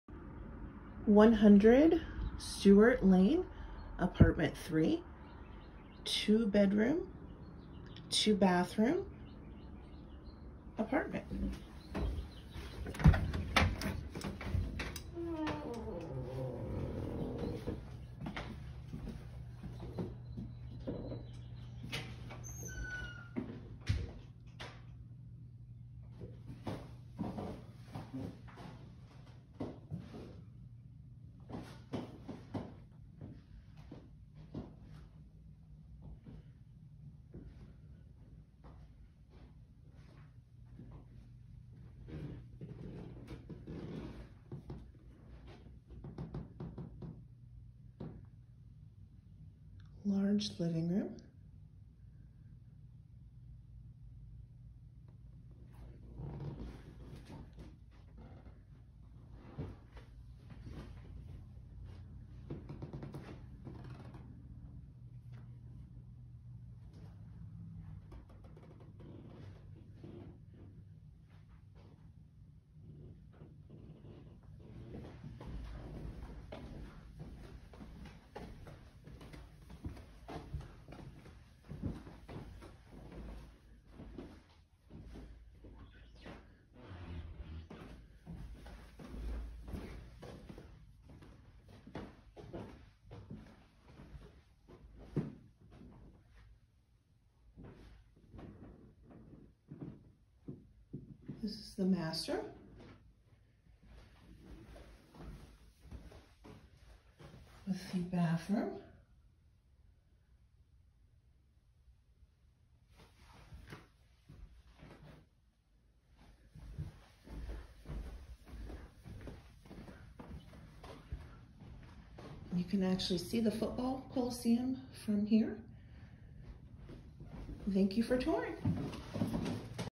100-3 Stewart Lane, Morgantown, WV 26505 Virtual Tour
This 2 bedroom, 2 bath apartment is located off of Stewartstown Road. The unit has a washer/dryer, dishwasher, deck, gas fireplace and carport. Tenants pay all utilities and garbage. No pets are allowed.